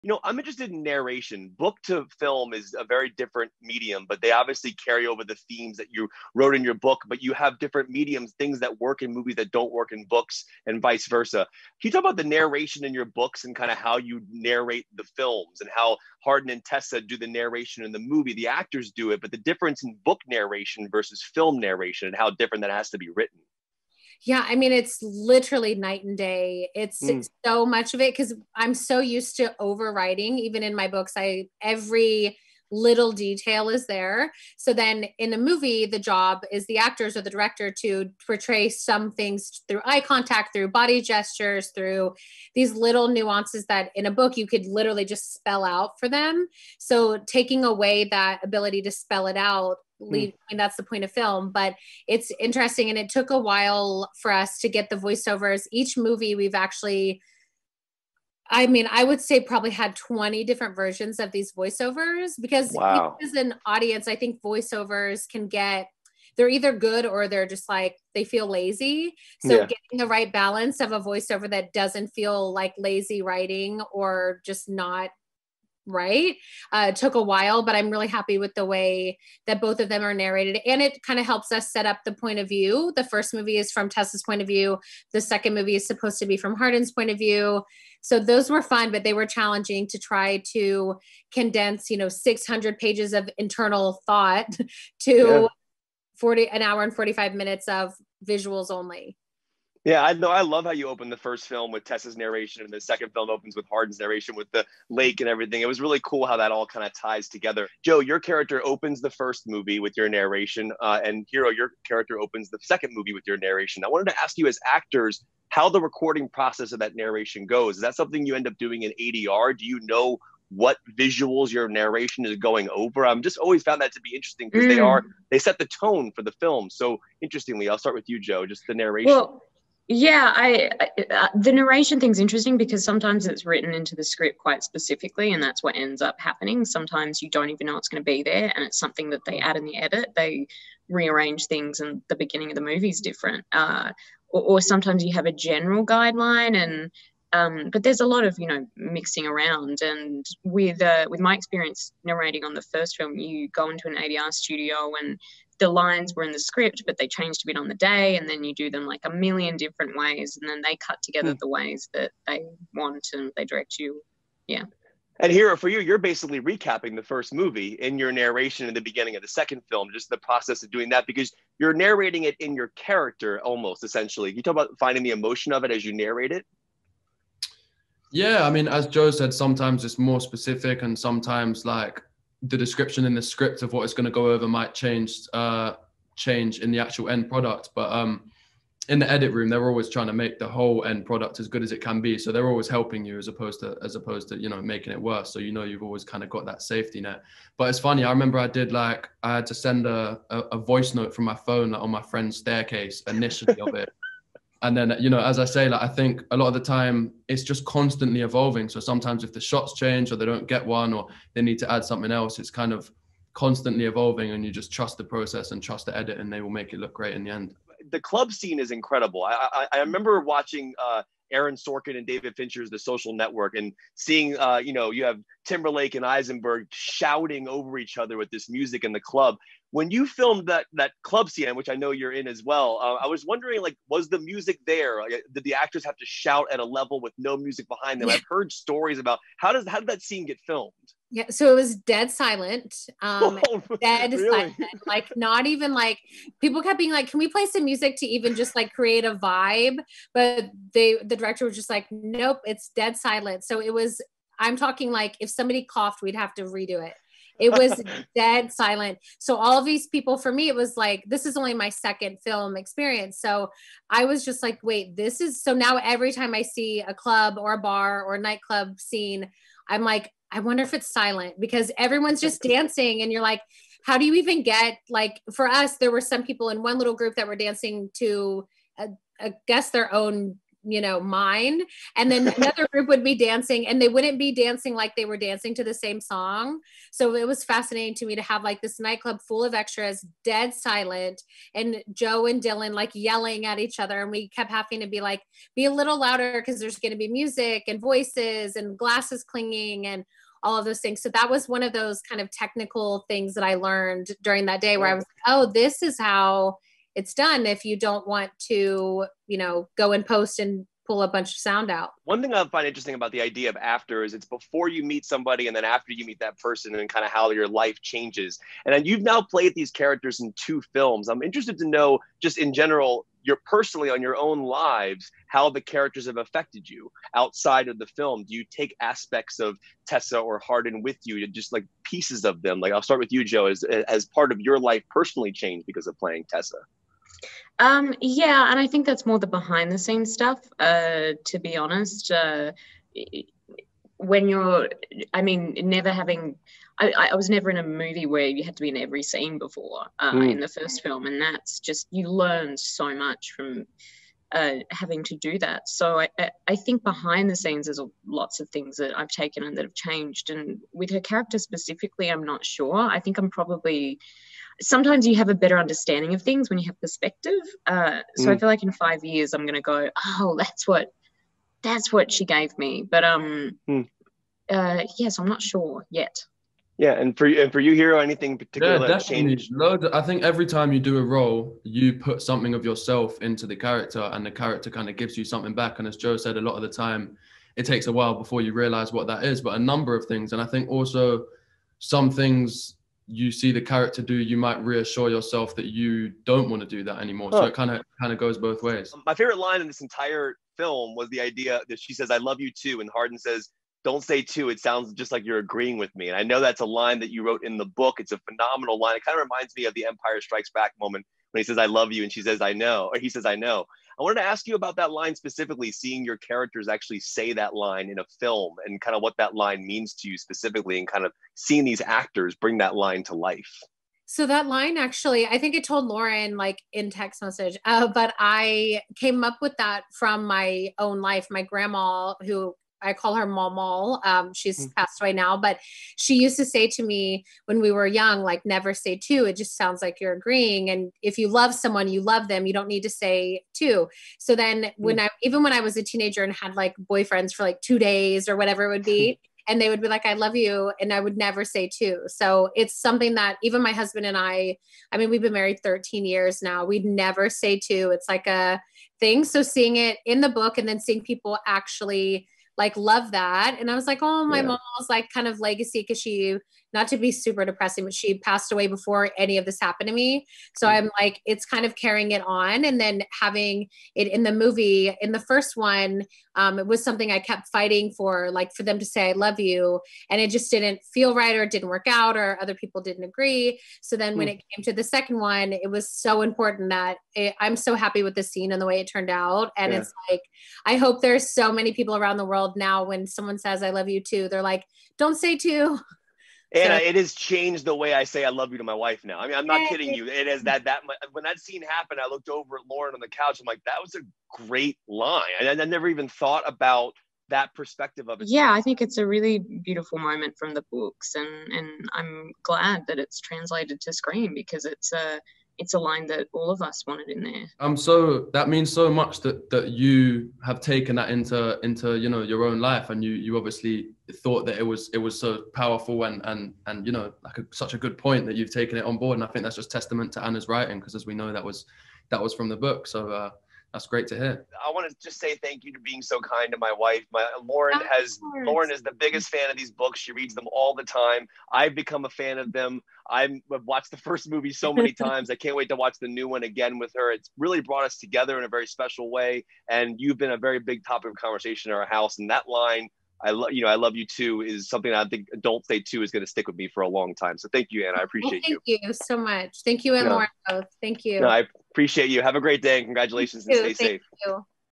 AFTER WE COLLIDED movie interviews - Anna Todd, Josephine Langford and Hero Fiennes Tiffin | FOX 5
Kevin McCarthy (Twitter/Instagram: @kevinmccarthytv) spoke with Anna Todd, Josephine Langford and Hero Fiennes Tiffin about "After We Collided". "After We Collided" is now playing in select theatres and via VOD. MORE